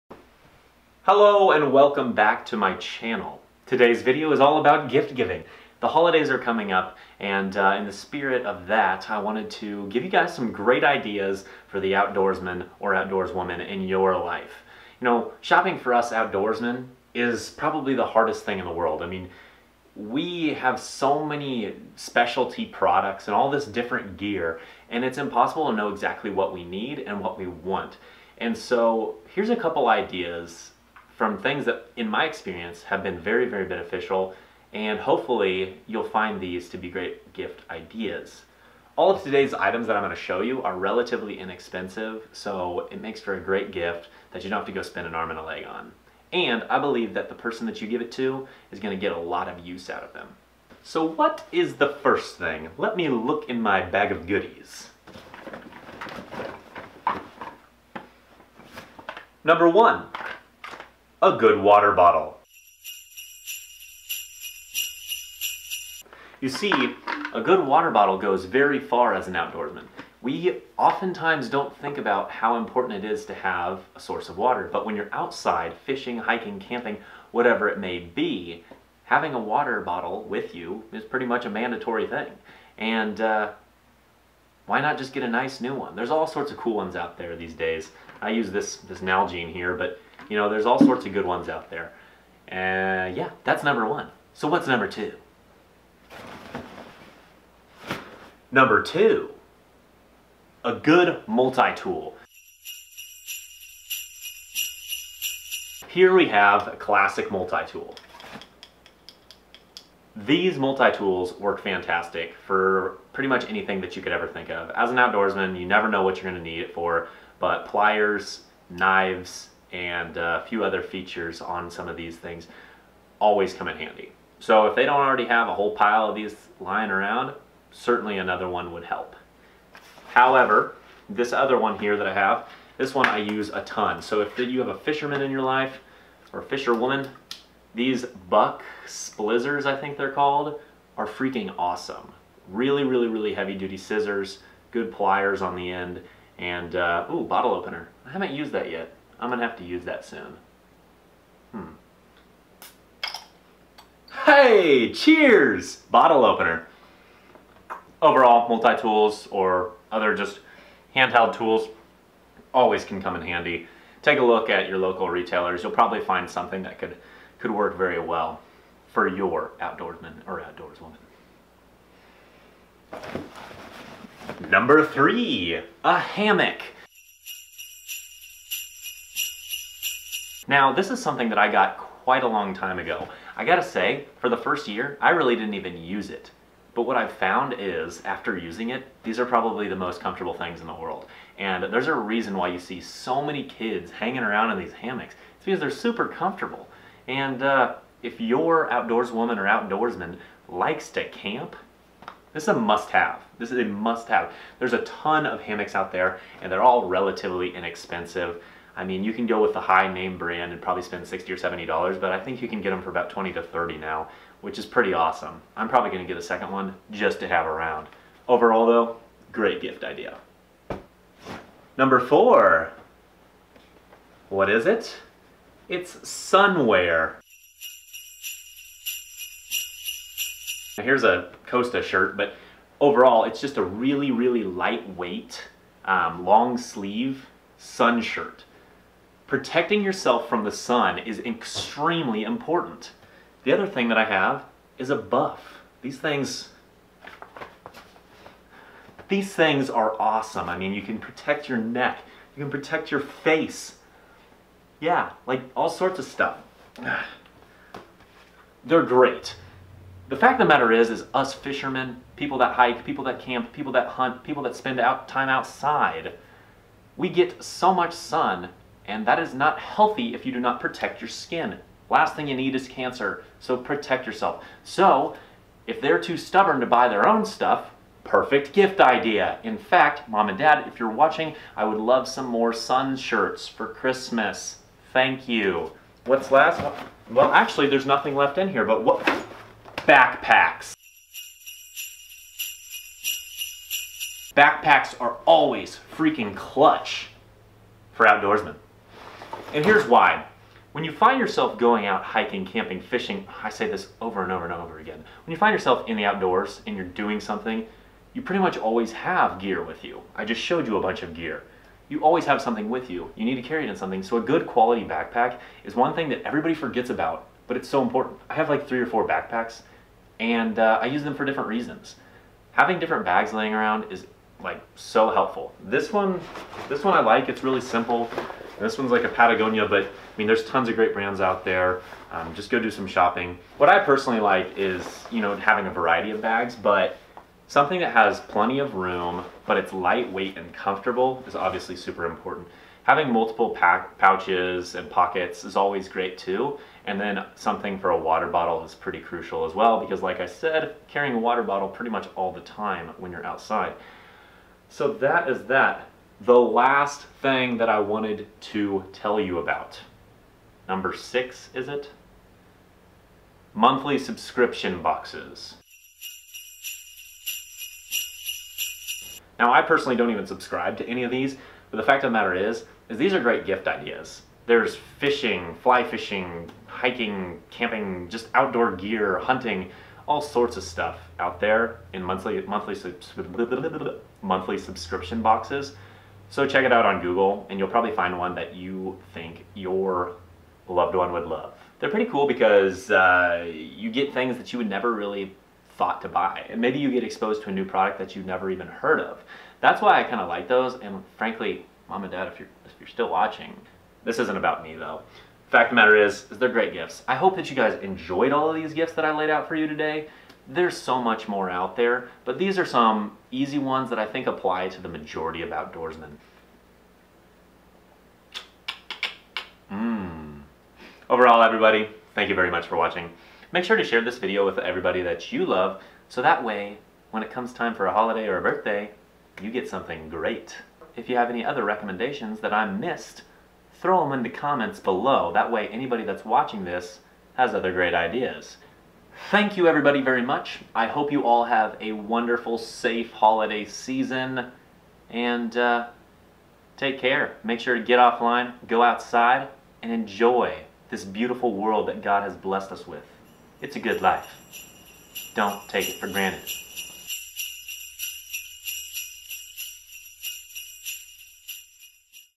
Gift Ideas for Outdoorsmen
What to get your loved outdoorsman / fisherman for Christmas??? These are a few ideas!

These are 6 great gift ideas for either birthdays or Christmas.
Gift #1 - 0:58
Gift #2 - 1:39
Gift #3 -  2:56
Gift #4 - 3:42
Gift #5 - 4:48
Gift #6 - 6:04

Water Bottle
Buck Splizzors
Try Amazon Prime Free